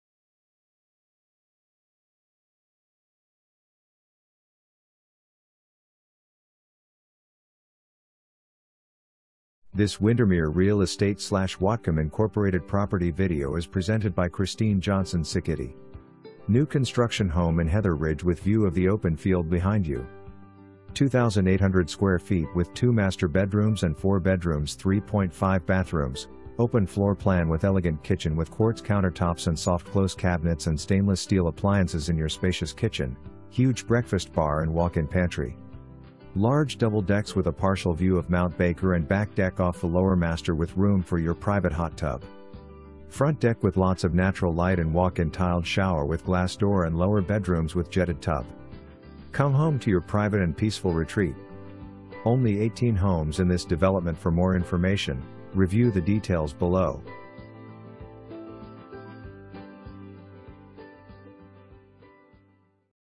Windermere Real Estate/Whatcom, Inc. - 2393 Hettervig Place
New Construction home in Heather Ridge with view of the open field behind you. 2800 sqft with 2 master bedrooms and 4 bedrooms 3.5 bathrooms, open floor plan with elegant kitchen with quartz countertops and soft close cabinets and stainless steel appliances in your spacious kitchen, Huge breakfast bar and walk in pantry. Large double decks with a partial view of Mt Baker and back deck off the lower master with room for your private hot tub. Front deck with lots of natural light and walk in tiled shower with glass door and lower bedrooms with jetted tub. Come home to your private and peaceful retreat. only 18 homes in this development

3 full bath, 1 half bath

Christine Johnson Cicchitti
Windermere Real Estate/Whatcom, Inc.